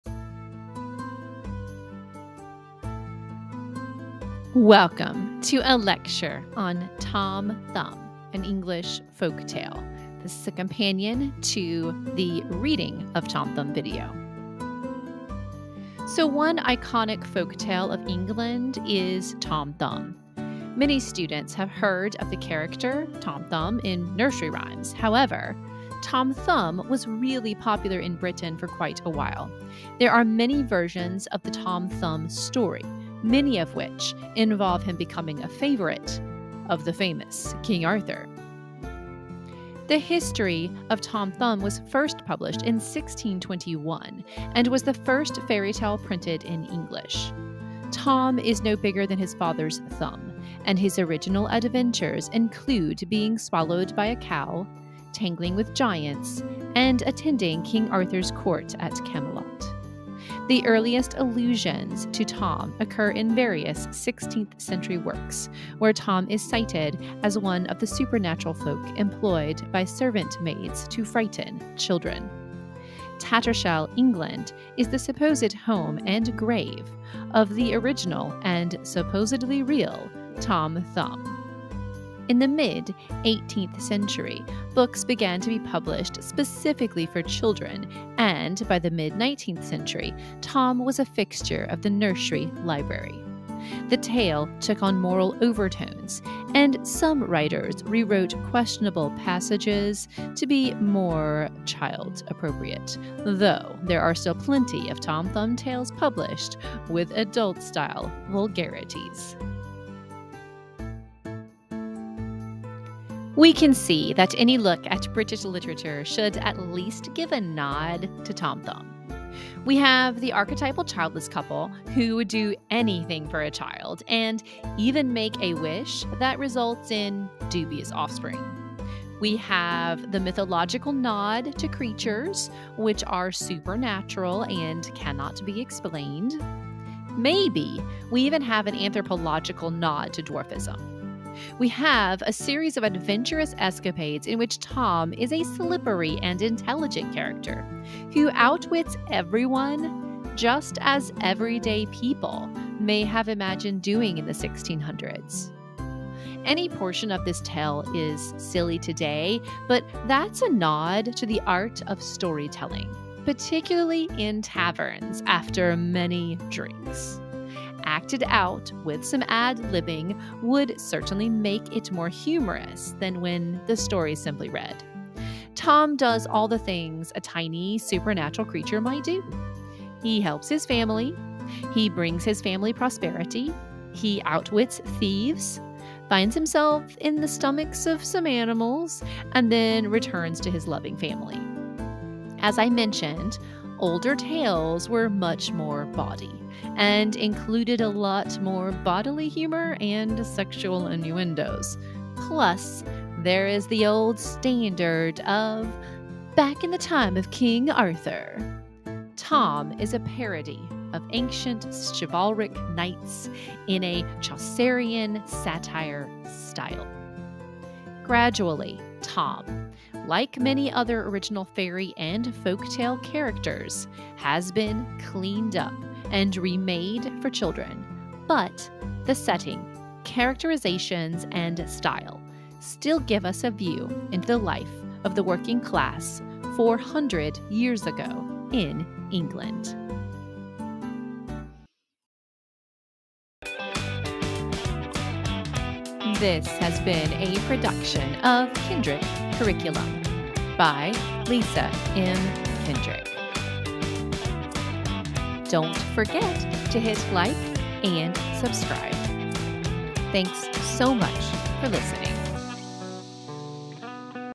Tom Thumb Lecture
A companion lecture to the reading if an anonymous version of the Tom Thumb tale.